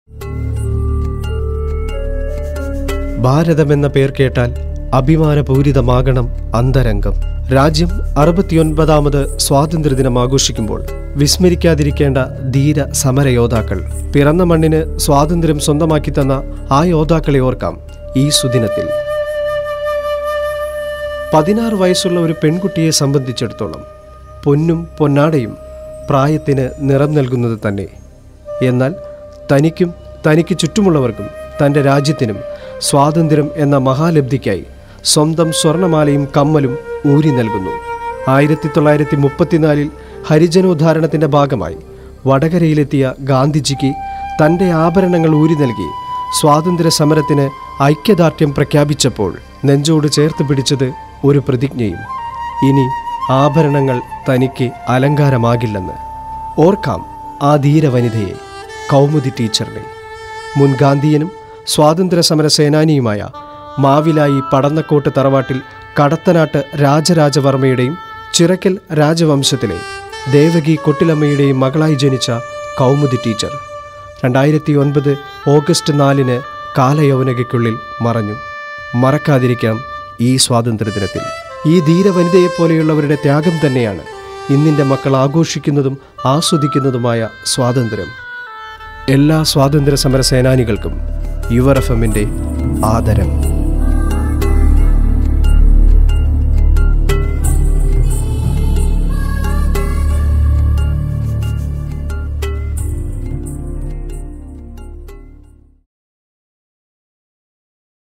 Freedom Fighter Stories - Kaumudi - a tribute by 104.2 Your FM Bahrain
As part of the on-air celebrations for 69th Indian Independence Day, 104.2 Your FM - Bahrain's only Indian radio station presents a few stories of India's freedom fighters; with their sacrifice, courage and determination India achieved independence on 15th August, 1947. We salute Kaumudi and all the freedom fighters who dared to dream of a country that we today proudly call, India!